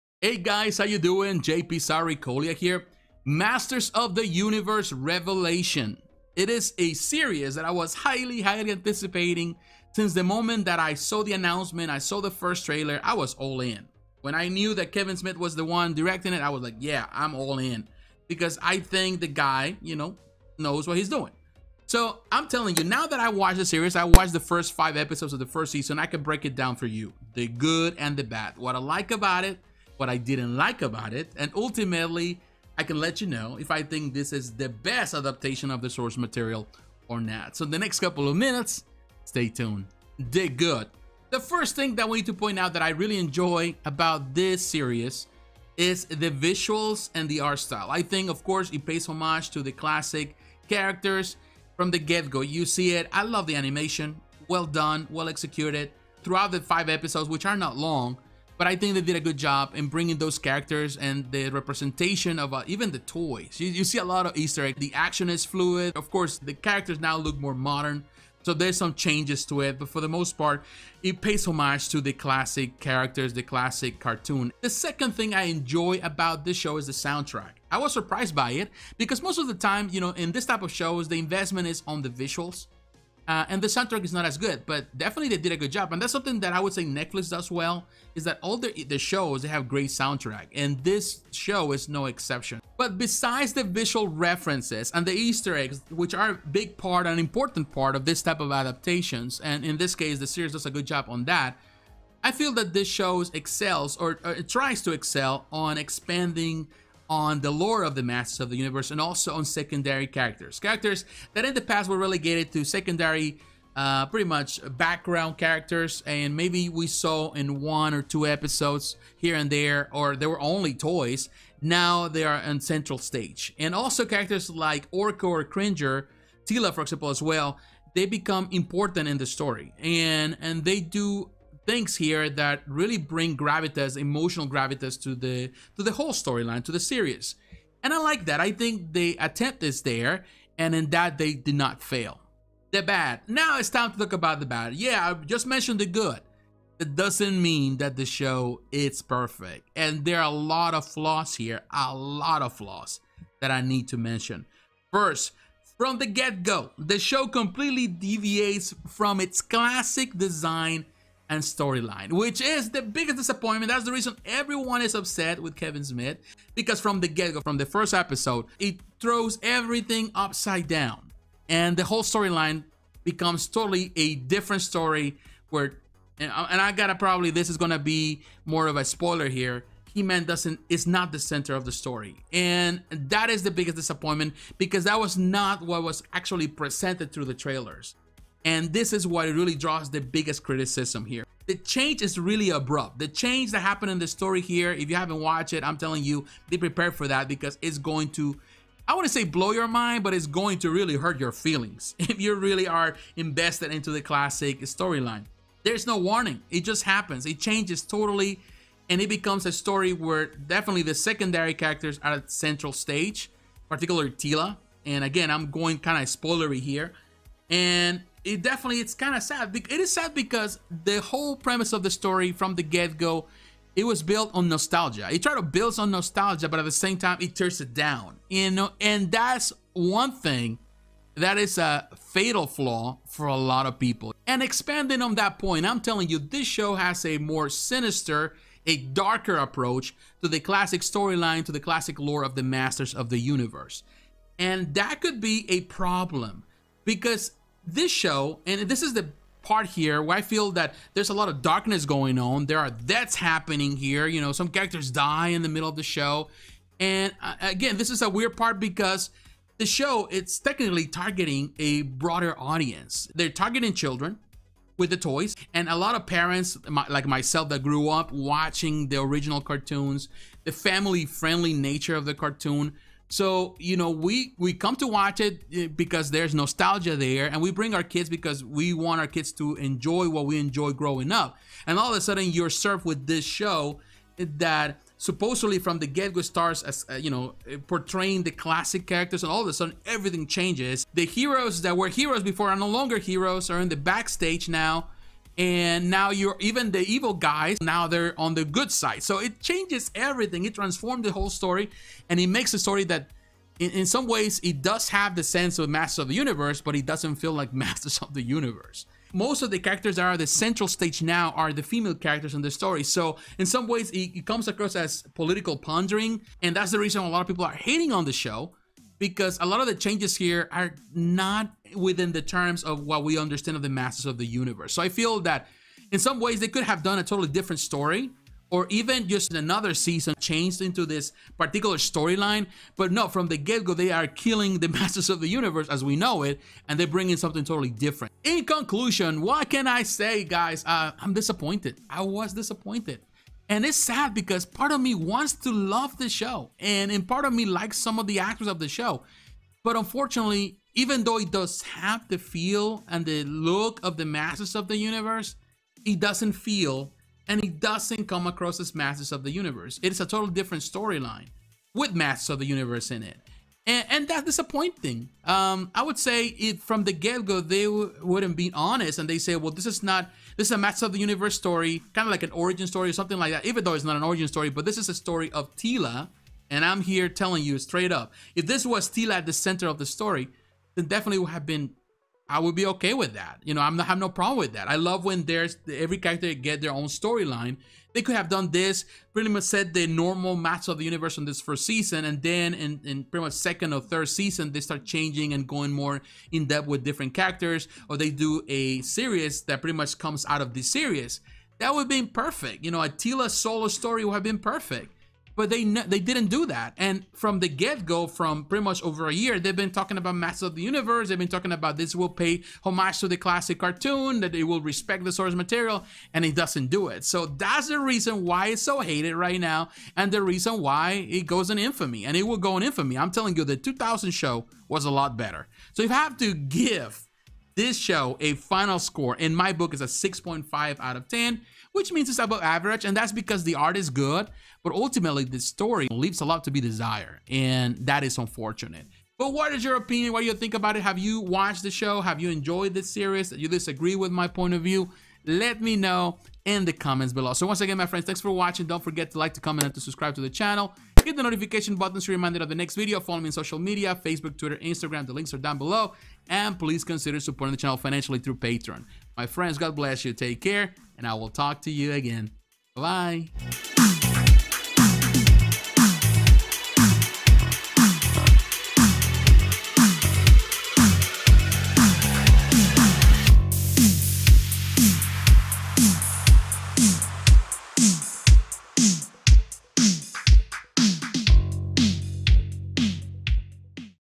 Masters of the Universe Revelation Review
Review of the 2021 Masters of The Universe Revelation Animated Series produced by Powerhouse Animation Studio, Mattel Television and distributed digitally through Netflix. Check it out!

Interested on sponsoring this channel or for advertisement to your product?

Walk with me on my spiritual journey at Born Again TV